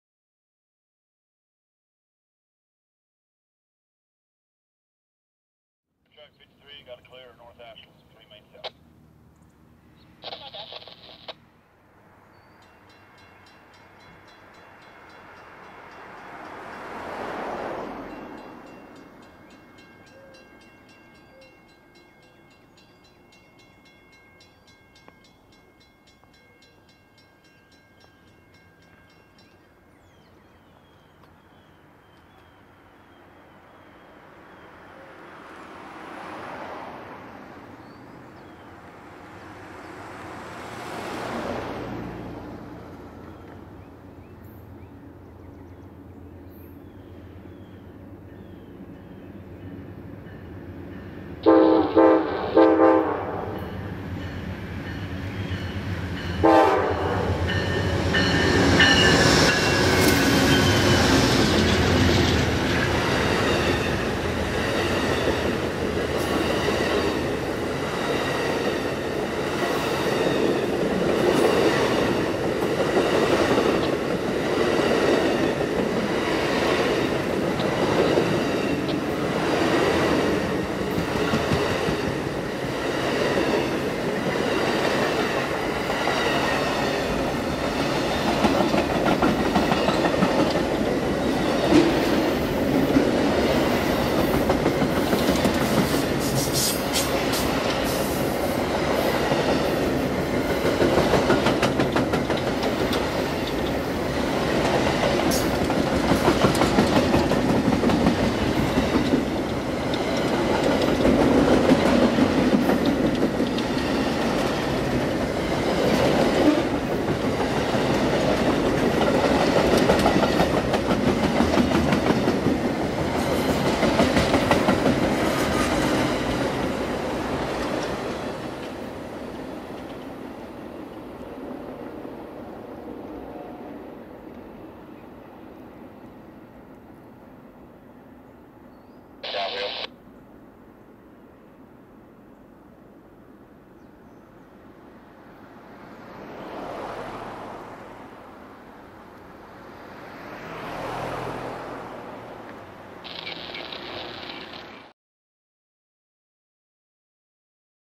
Amtrak's Auto Train 53 Ashland, VA 3 17 17
Last up was the Auto train, which I had come out to catch. Train 53 today had P40 830 and P42 17 leading the long line of Superliners and Auto carriers. One of the engineers today was Ryan Riddell, who can be seen waving from the left seat as the engines pass. Also further back you can see Michael Brandon waving from the door window of a sleeper. He had asked me to shoot this train as he was riding. He told me that the car attendant was with him when he had the window open to wave.